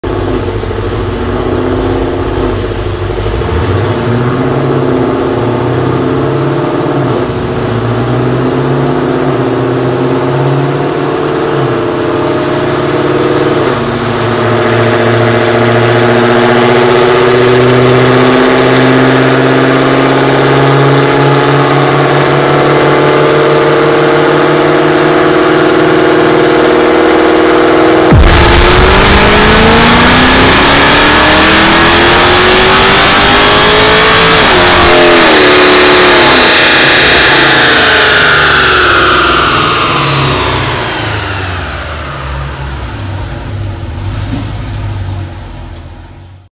HHPracing Stage 3 Heads & Cam on 392 HEMI
What can HHPracing's stage 3 heads and upgraded cam do for your 392 Hemi? Nearly 500 whp, that's what. 😈